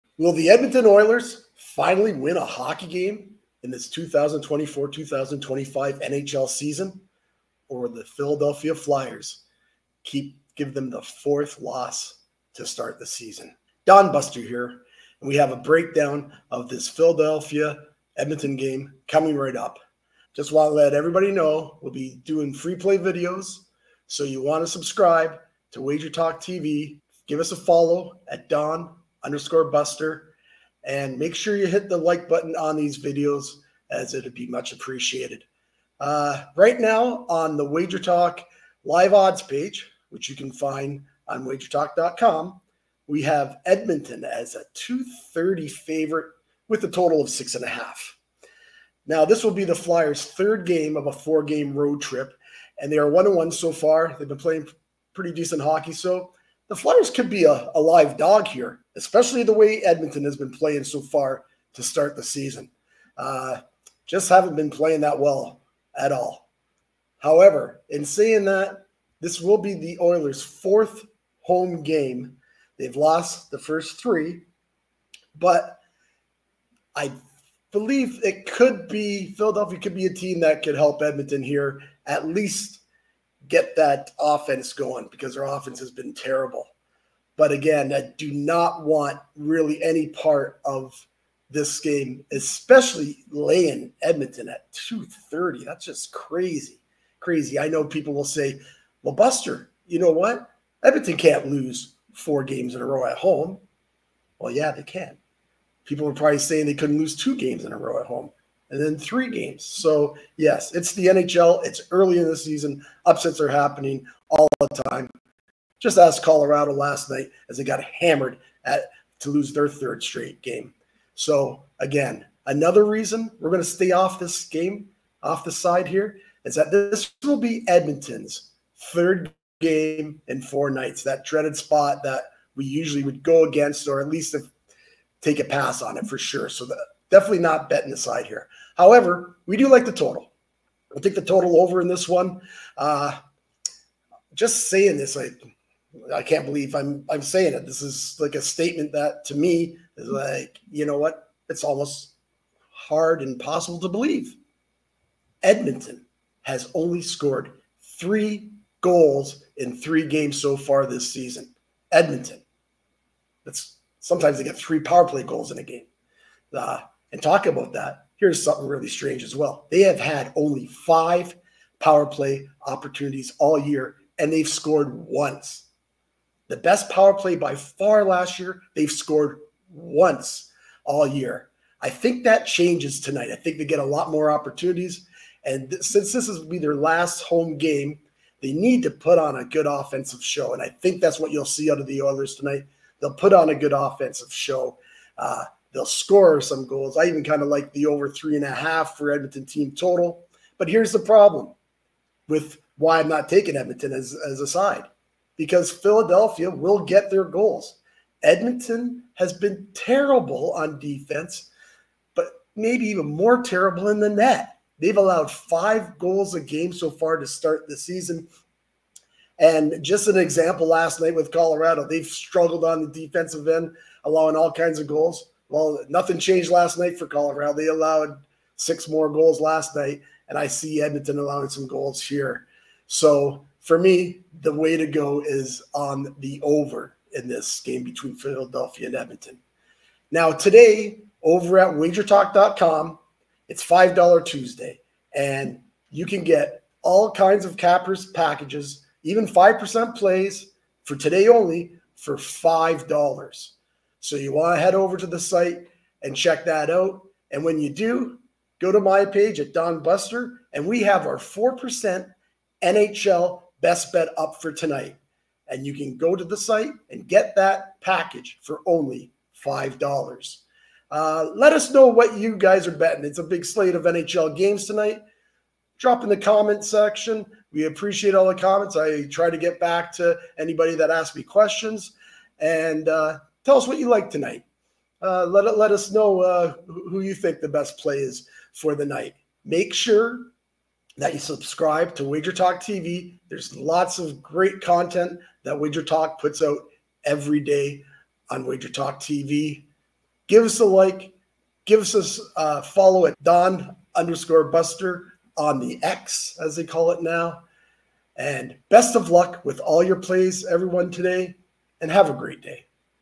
Philadelphia Flyers vs Edmonton Oilers Predictions and Picks | 2024 NHL Bets 10/15/24
🥅 If you are looking for NHL picks and predictions for the Philadelphia Flyers vs Edmonton Oilers matchup today, we have you covered! Our NHL handicapping experts discuss the opening weekend from a sports betting perspective, sharing their picks and predictions vs. the current odds.

Philadelphia Flyers vs Edmonton Oilers - NHL Betting Preview
🗓️ Date: Tuesday, October 15, 2024
🕖 Time: 10:00 PM ET / 7:00 PM PT
📍 Location: Rogers Place in Edmonton, AB
📺 TV Broadcast: ESPN

Teams:
Philadelphia Flyers
Edmonton Oilers

Betting Info:
Spread: Oilers -1.5 (+135)
Total (Over/Under): 6.5